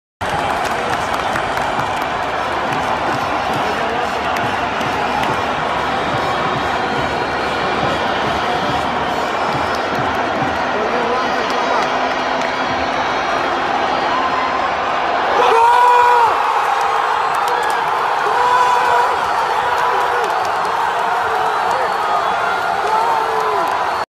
El Golazo de Tiro libre de Juan Fernando Quintero en el Mundial, desde la tribuna
Mundial de Rusia
Copa Mundial de Rusia 2018
Falcao
Juan Fernando Quintero